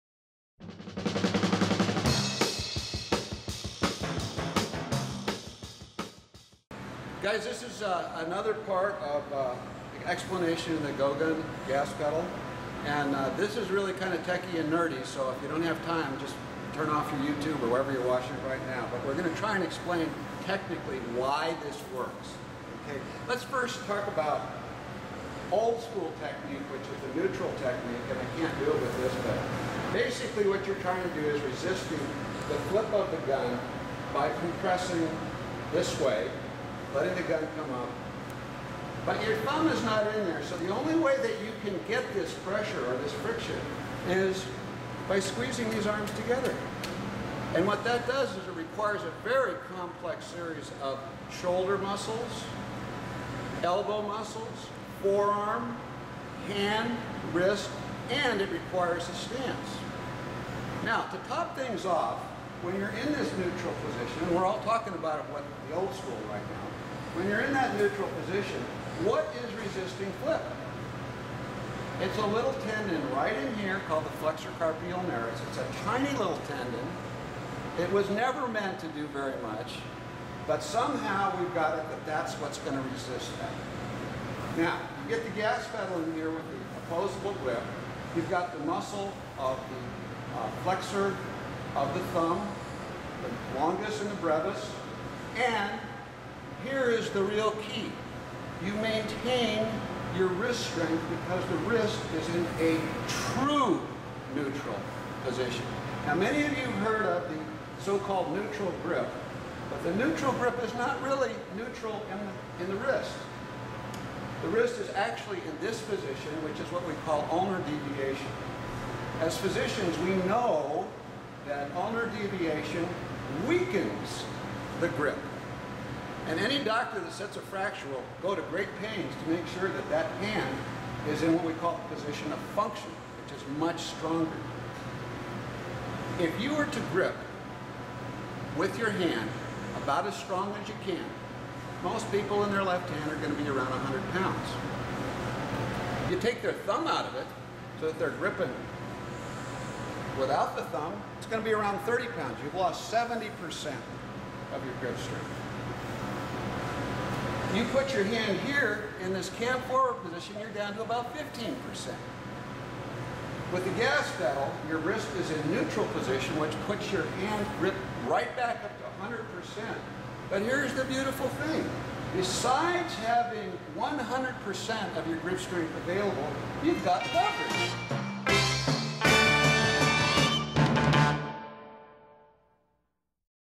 Tech explanation How the Gas Pedal Works.wmv
A better Gun Grip for .45 acp, technical explanation How the Gas Pedal Works to make it easy to control recoil and faster aquisition.